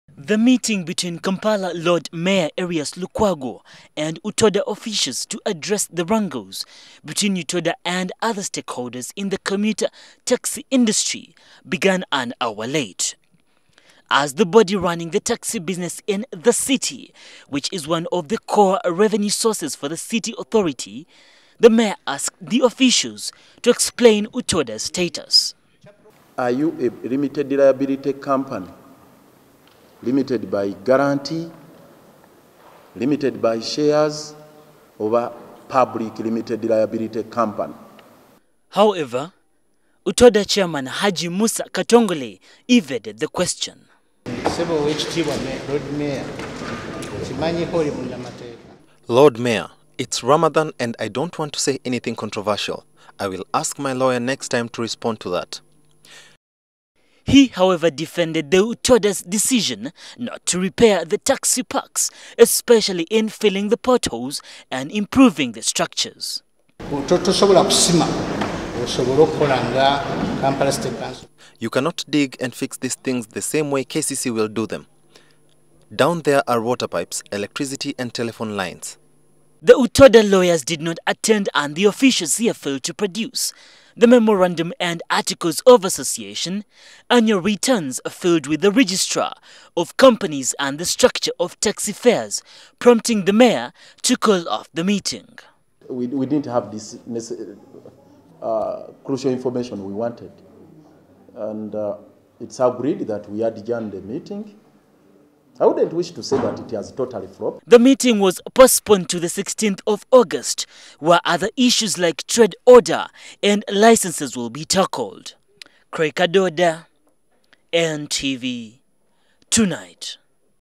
Lukwago meets UTODA over contentious contract
Kampala Lord Mayor, Erias Lukwago, has directed Uganda Taxi Operators and Drivers Association (UTODA) to present documents to justify its management of the city's commuter taxi business, which has lately been under scrutiny. Lukwago who held a brief meeting with UTODA officials at City Hall earlier today, cut short the engagement when he realized the officials had not been accompanied by their lawyer.